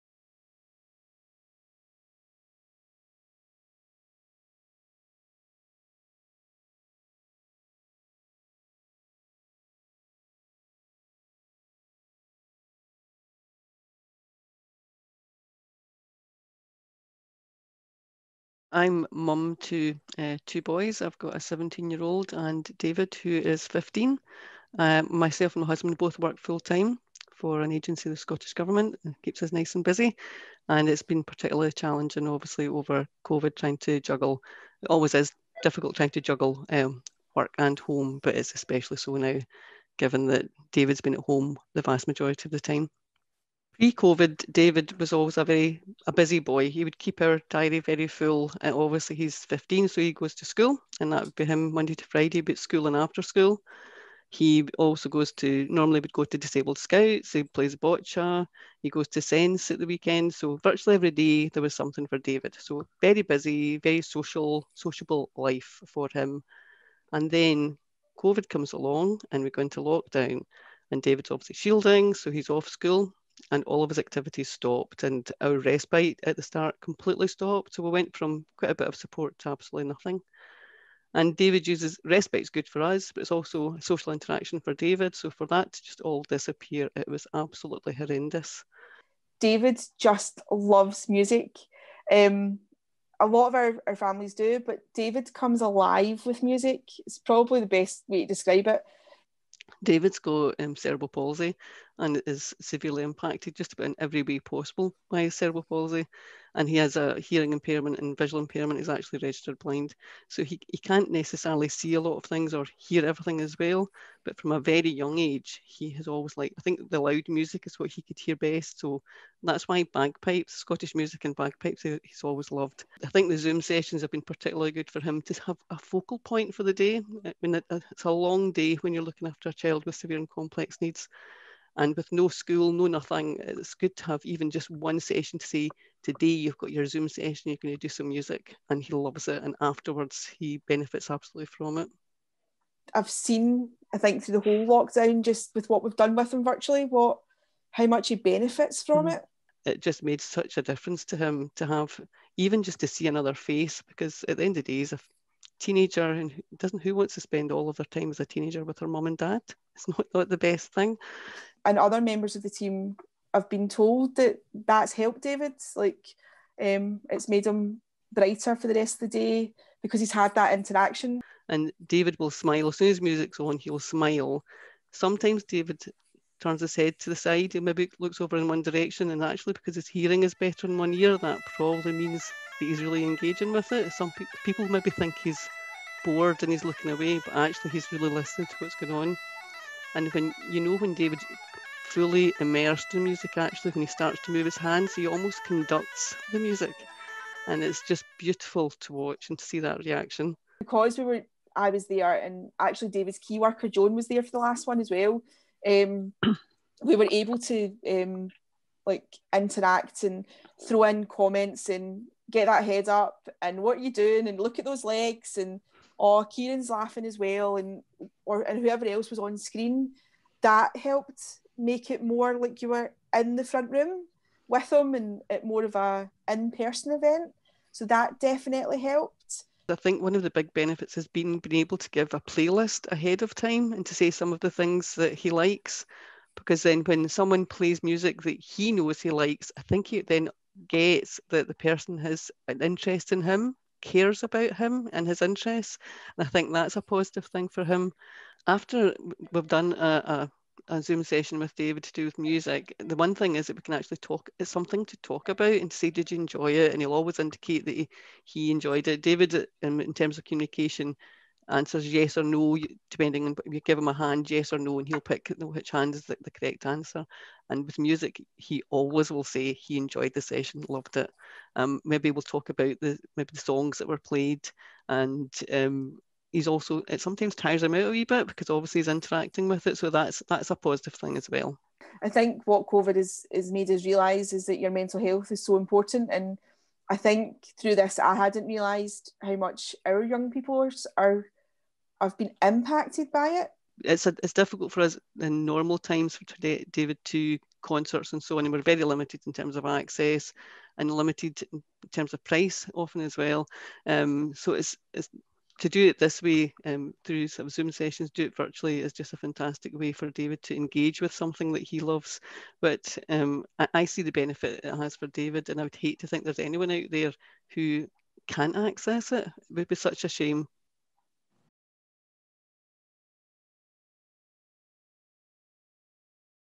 David's Story
David is 15 and accesses respite care at Robin House in Balloch, West Dunbartonshire, which is operated by the charity Children’s Hospices Across Scotland (CHAS).

He loves music and has recently joined some of our MusicEveryDay live stream sessions. We spoke to David’s mum Evelyn and CHAS Activities Facilitator Alison on a video call.